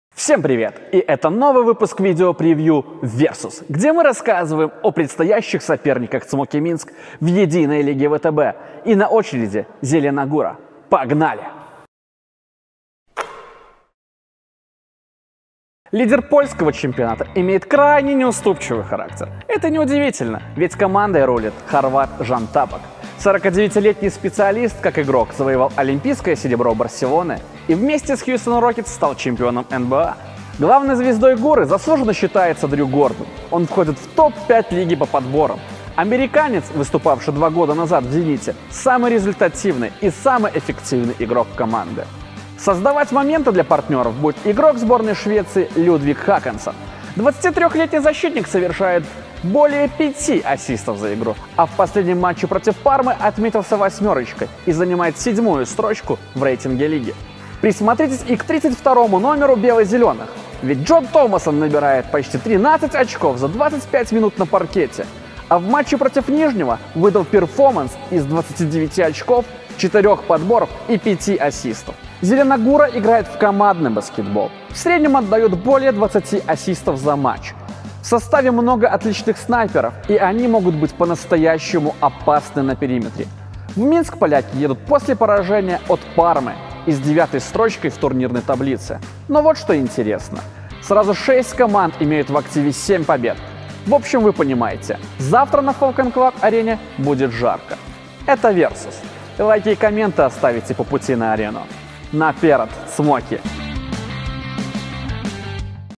Versus. "Зелена Гура"
Новый формат превью, в котором мы рассказываем о предстоящем сопернике "Драконов".